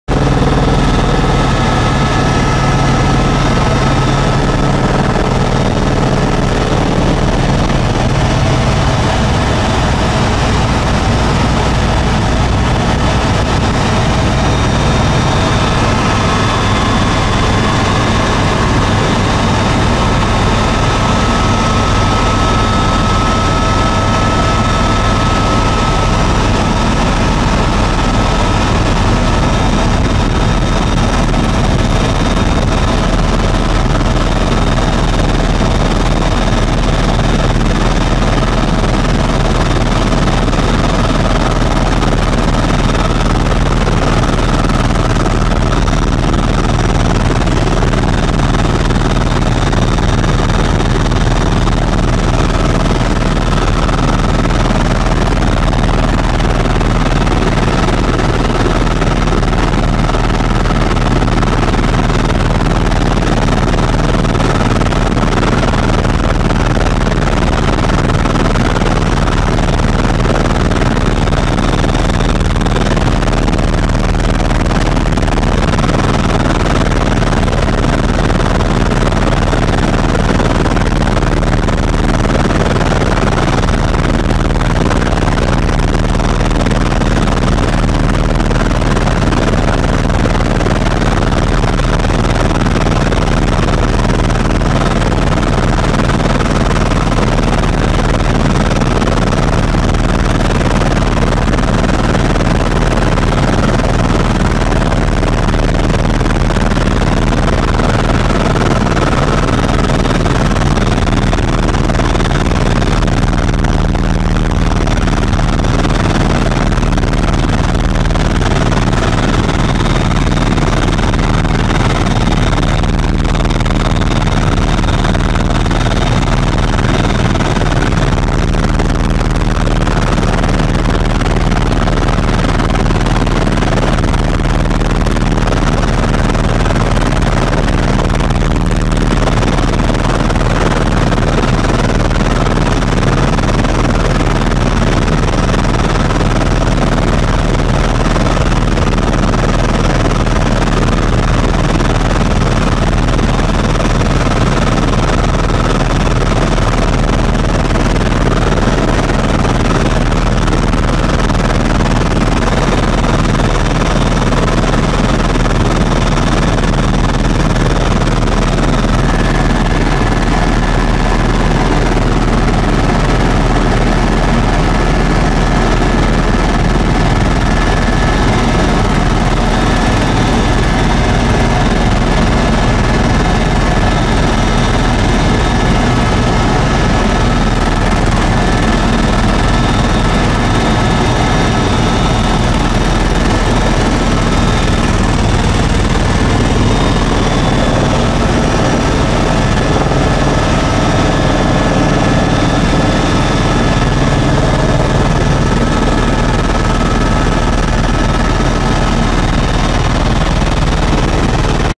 Helicopter Trip Las Vegas Strip June 2011
Clip of Helicopter trip heading south down strip back towards McCarran Airport in a ECO Star Eurocopter from Maverick Helicopters